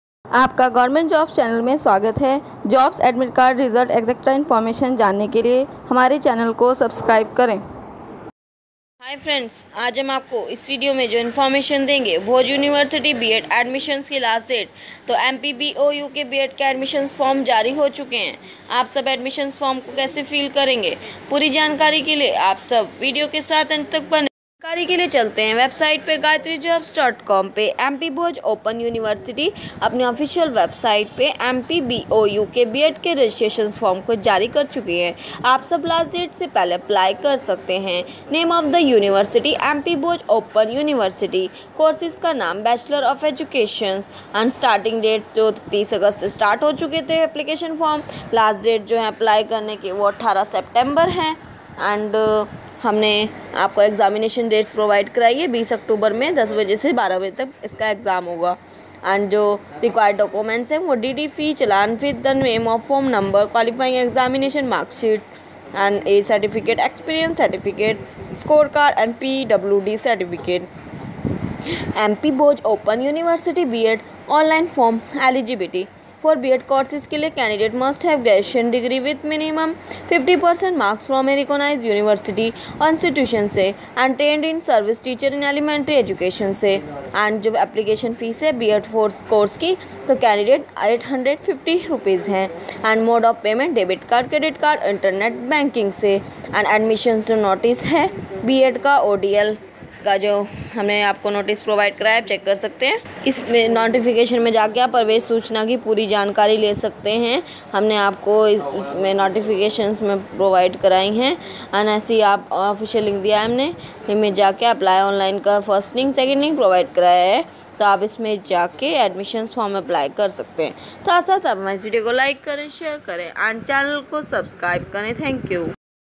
MPBOU B Ed Admission Form 2019 Last Date, Eligibility, Exam
MP Bhoj Open University has released the MPBOU B Ed Admission Form 2019 for the candidates who want to take admission in Bachelor of Education Course.